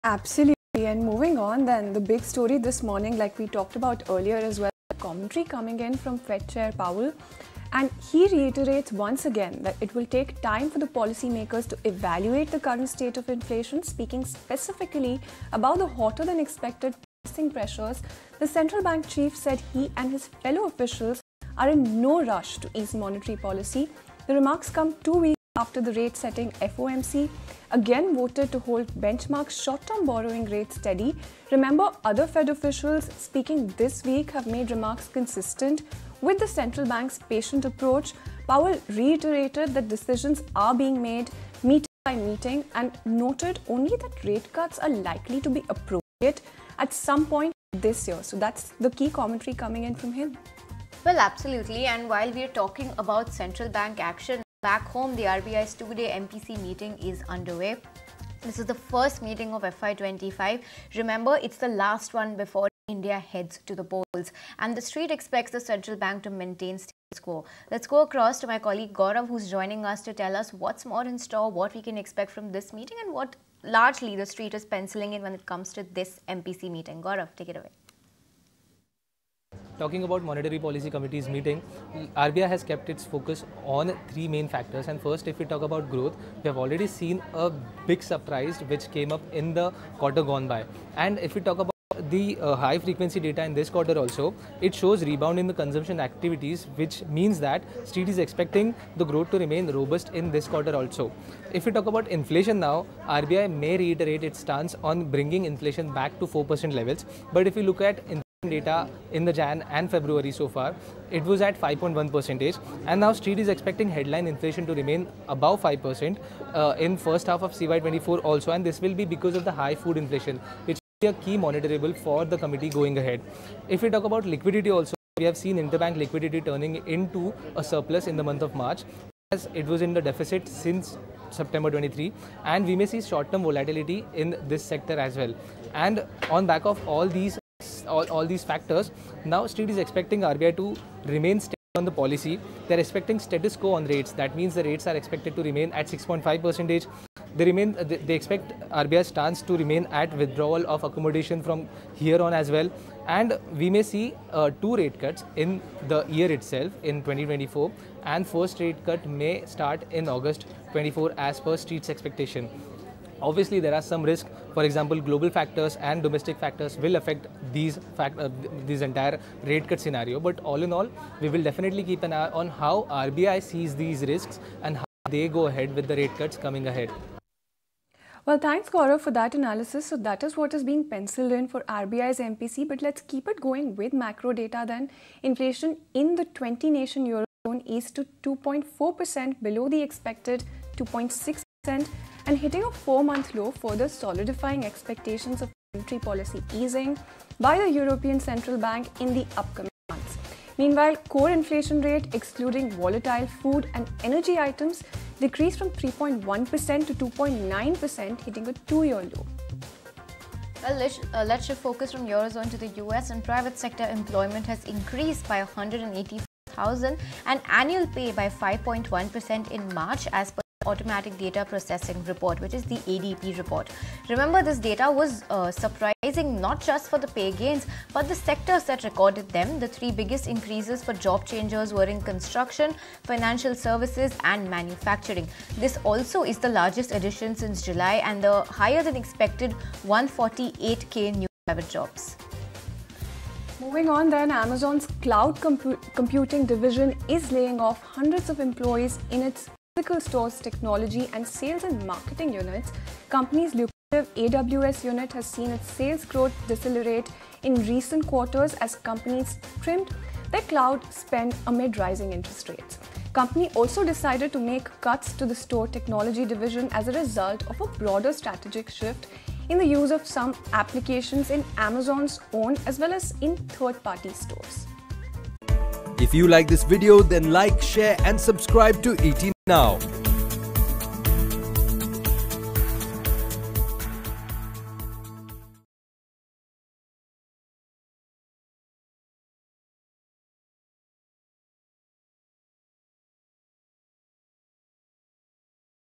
Jerome Powell Reiterate Rate Cuts Later This Year; RBI MPC Meet In Focus & More | Global News
Jerome Powell Reiterate Rate Cuts Later This Year, Fed Takes Wait & Watch Approach RBI MPC Meet In Focus & More | Global News